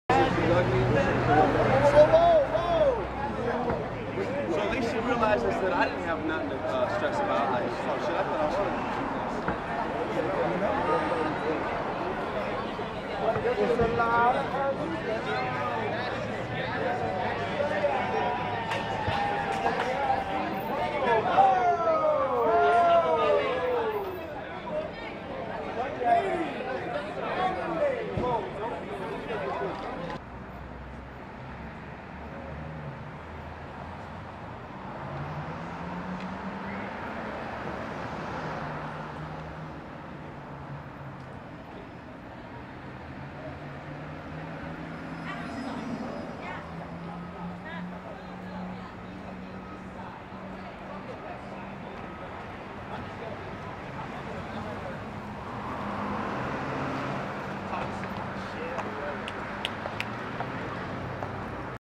Brittany Snow and Date with Friends depart Bootsy Bellows...
Brittany Snow and Date with Friends depart Bootsy Bellows in West Hollywood at Celebrity Sightings in Los Angeles Brittany Snow and Date with Friends depart Bootsy Be on August 03, 2012 in Los Angeles, California